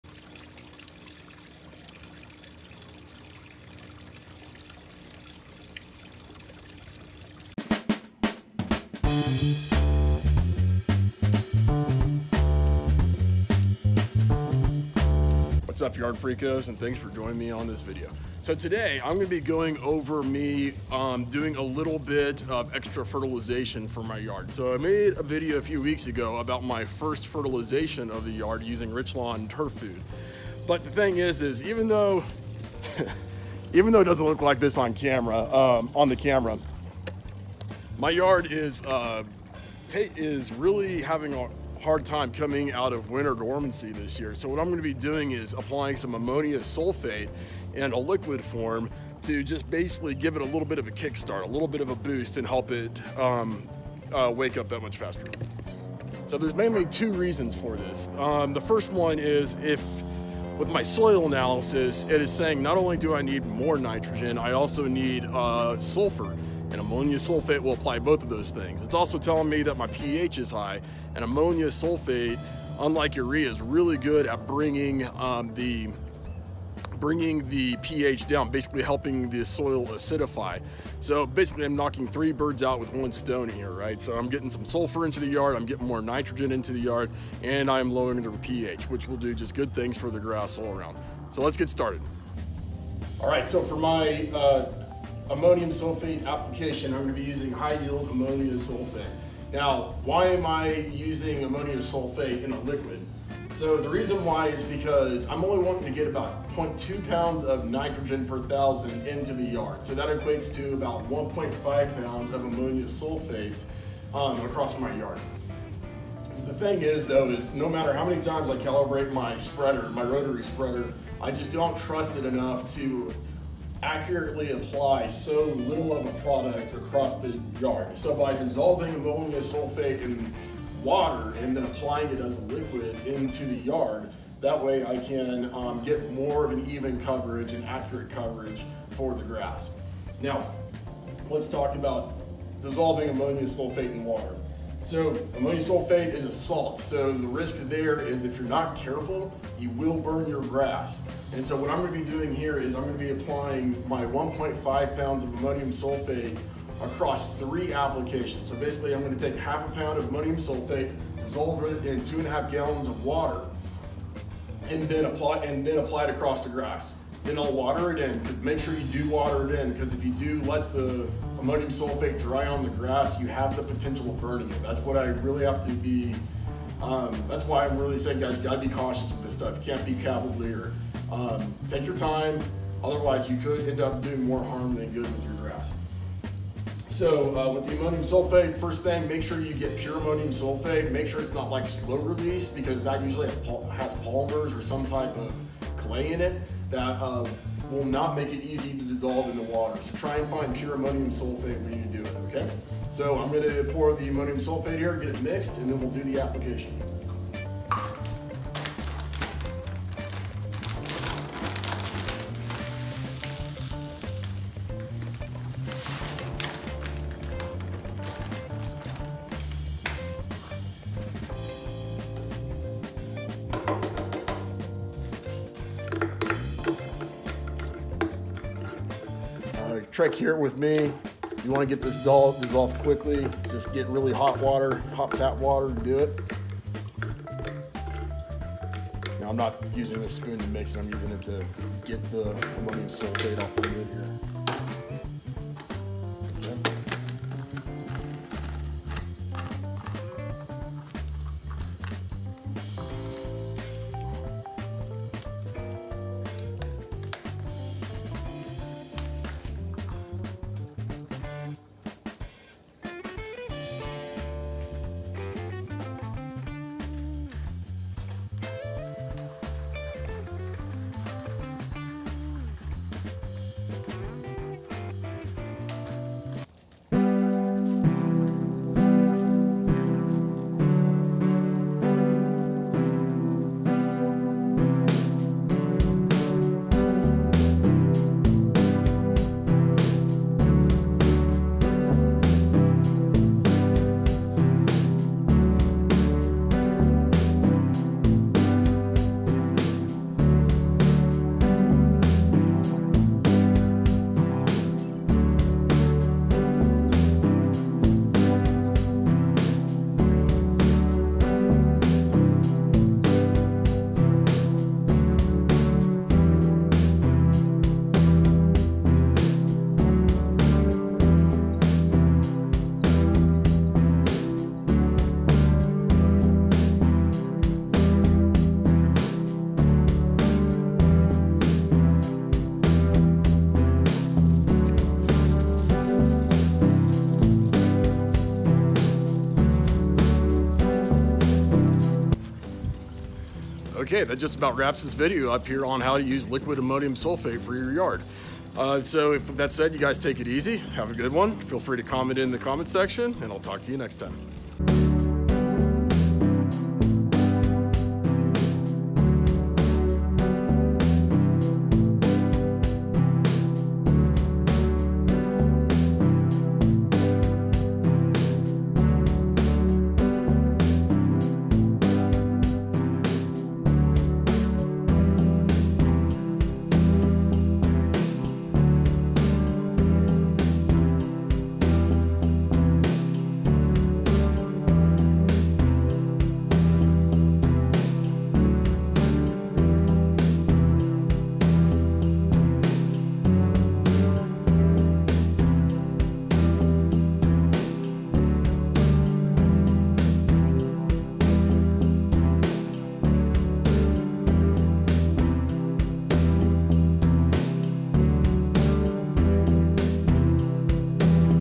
How to Apply Liquid Ammonium Sulfate to Your Yard
Give your lawn an extra kick using liquid ammonium sulfate.

This video discusses how to dissolve ammonium sulfate into water, and then apply it gently to your lawn to avoid salt burn.

Ammonium sulfate is great for nitrogen, sulfur, and ph needs for your lawn. I highly recommend it for either spoon feeding your grass, or as a general fertilizer mixed with other sources.